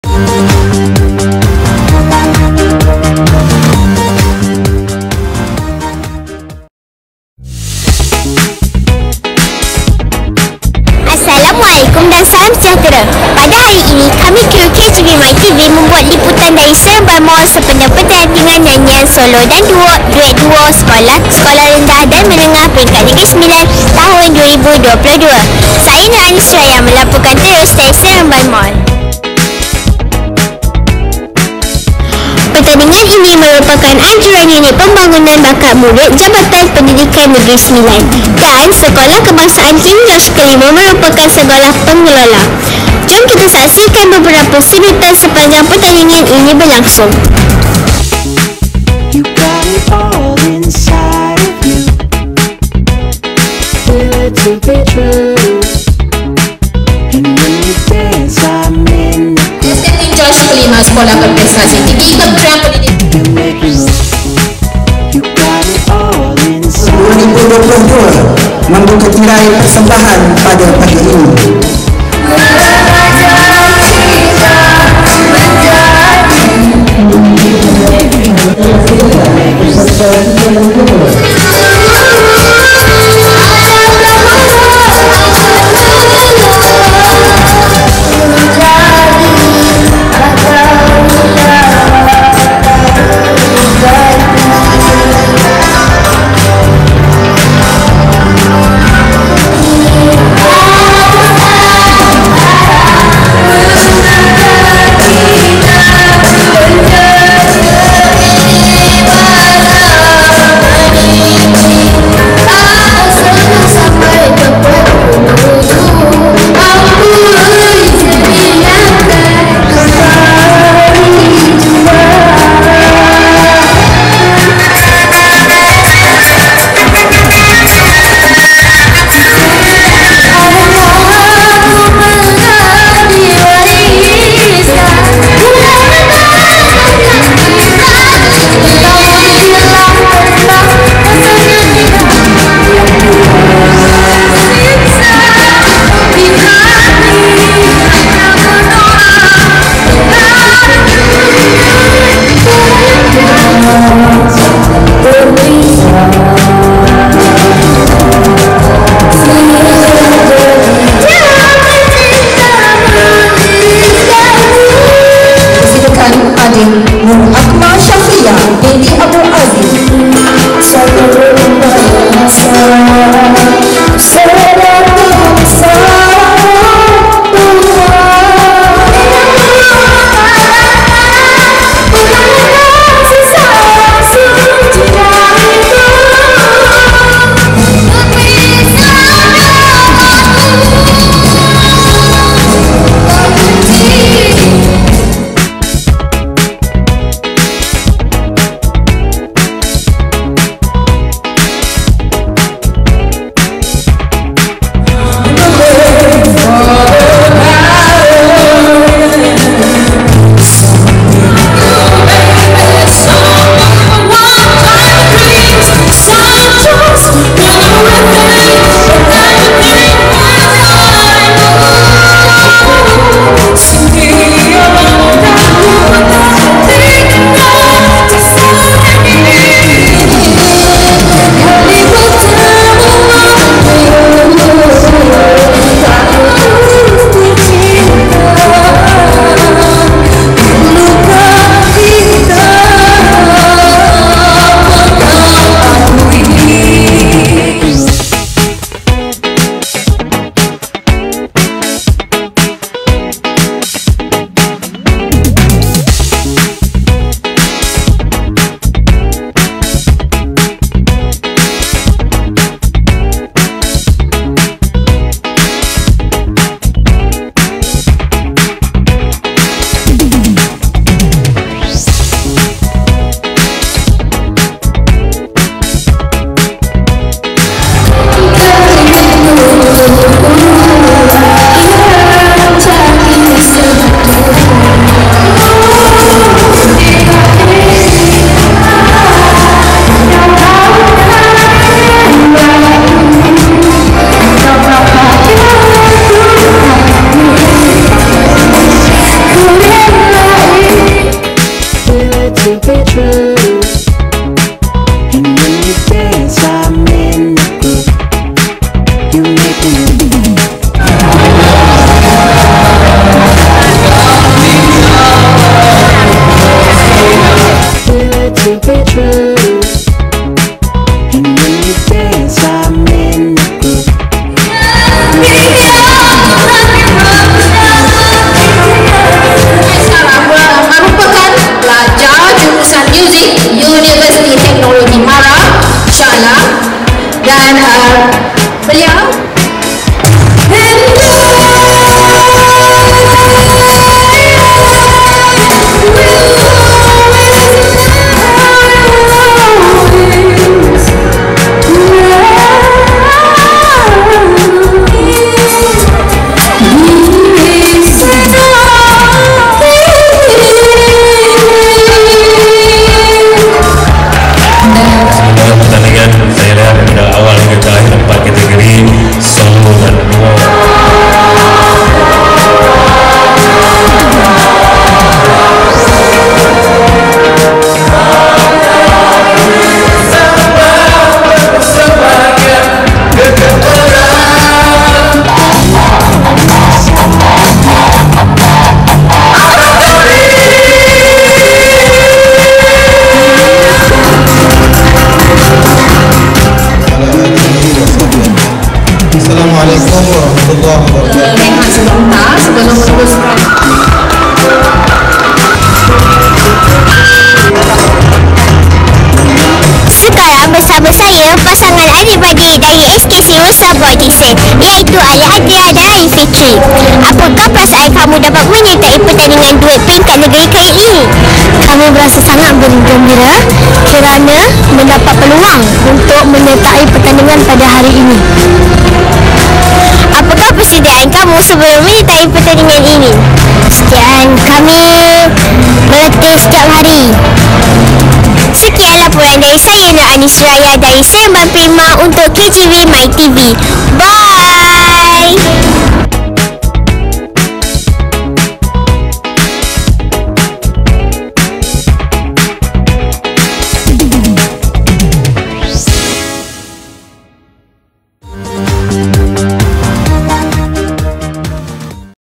EPISOD 033 PERTANDINGAN NYANYIAN SOLO & DUO/DUET MINUS ONE
Pertandingan Nyanyian Solo dan Duo / Duet Minus One Tahun 2022 telah diadakan pada 7 Ogos 2022 di Seremban Prima. Pertandingan Peringkat Negeri ini telah dikelolakan oleh SK King George V, Sekolah Berprestasi Tinggi TS25 KPM.